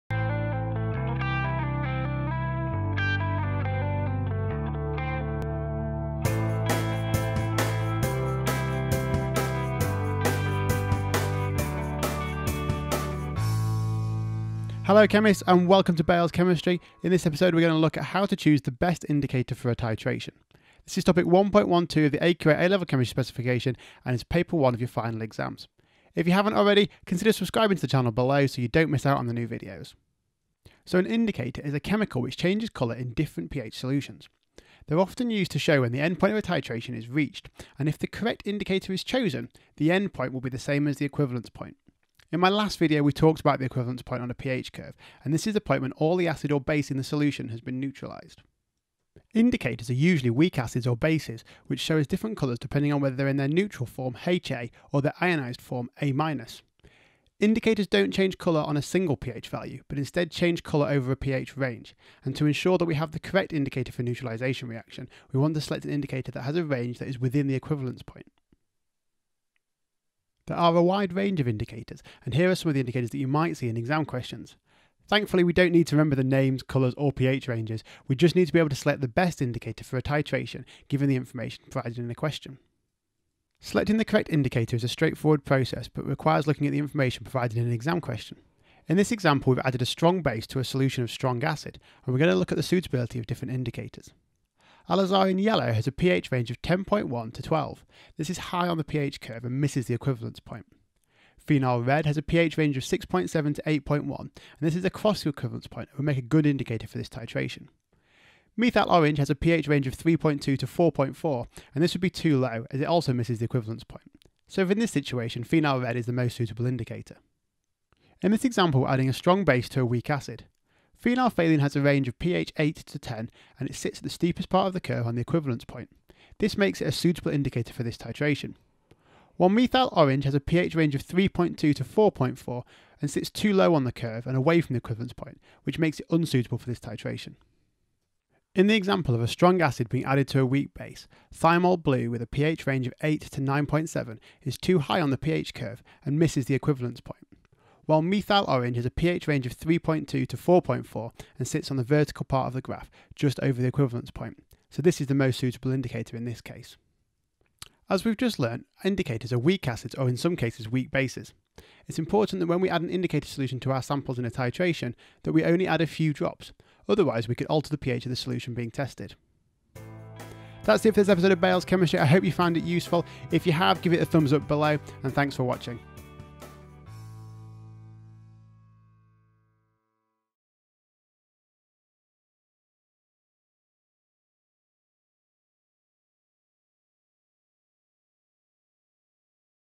Choosing an Indicator, Paper 1 - AQA A Level Chemistry
In this episode I explain how to correctly select an indicator for an Acid Base Titration. This is 1.12 Acids and Bases on the AQA A Level Chemistry Specification. Episode Sections Below...

0:00 Intro
0:30 Indicators
1:08 Table of Indicator values
1:22 Selecting an Indicator